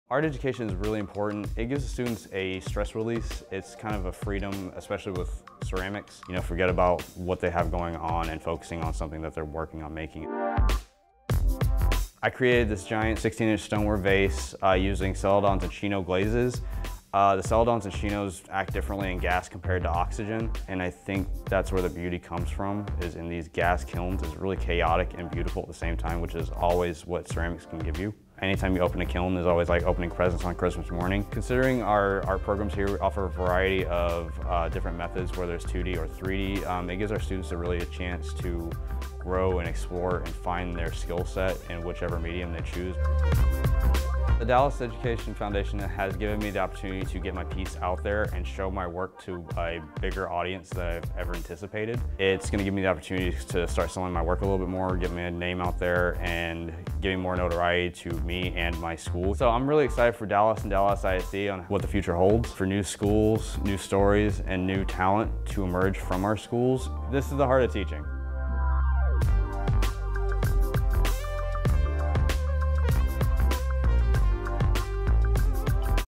HeartofTeaching
Tracy King of Dallas ISD's W.T. White High School shares his passion for ceramics with our students every day. His art will be available for auction at the Dallas Education Foundation's Heart of Teaching exhibition and gala.